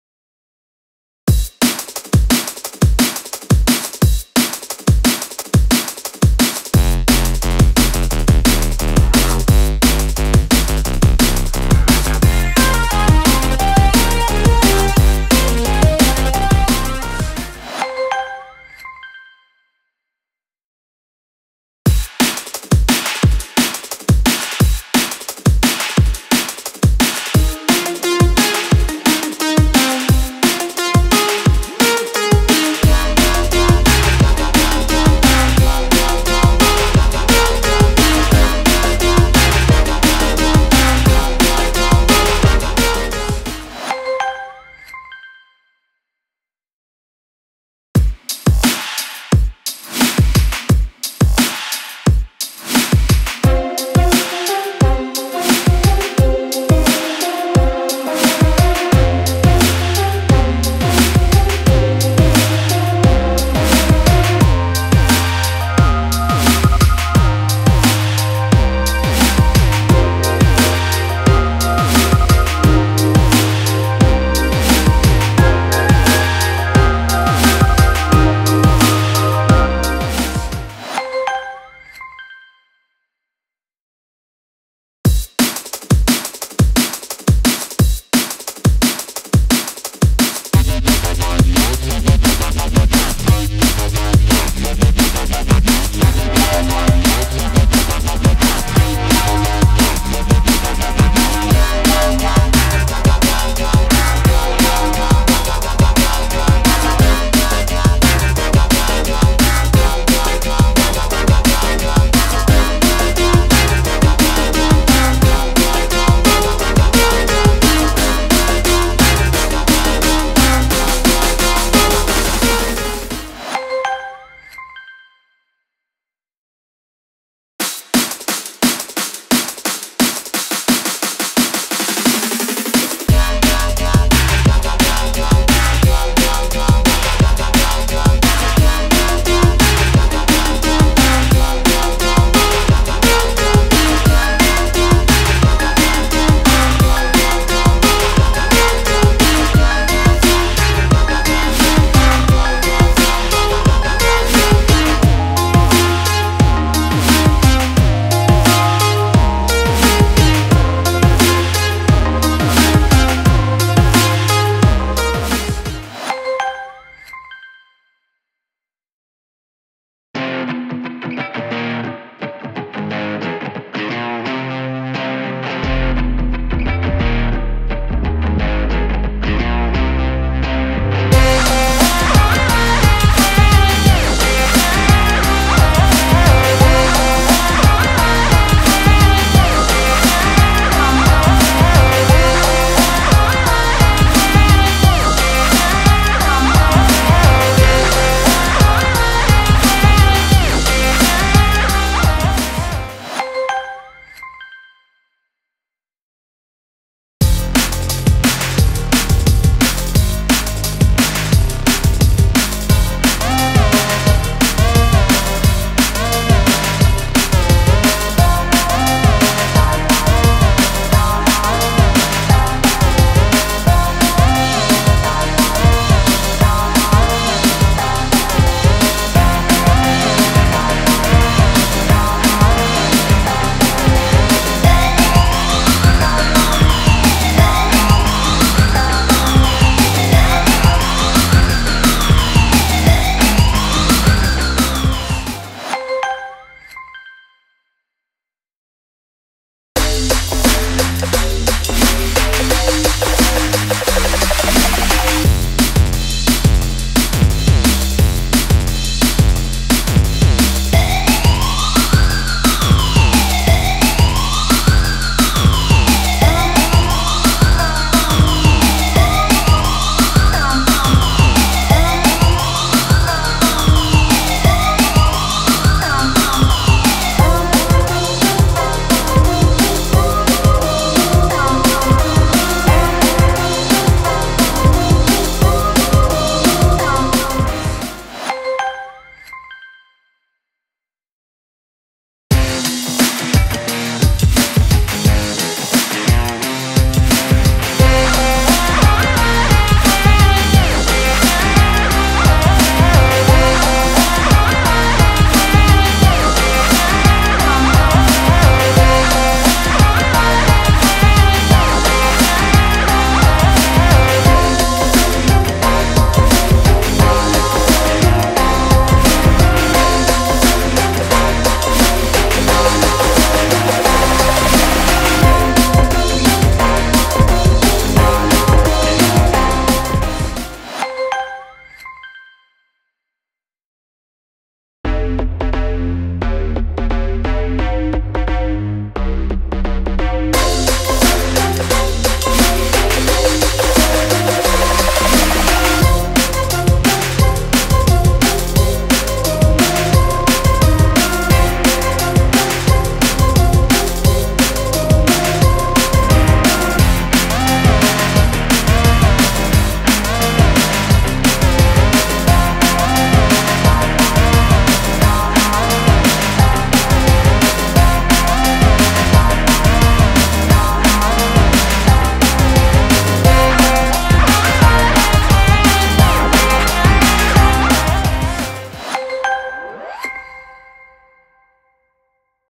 Broken Bounce Groovepad Lessons
In this lesson I use the sound pack named Broken Bounce from the app Groovepad then I follow through each lesson from 1 to 10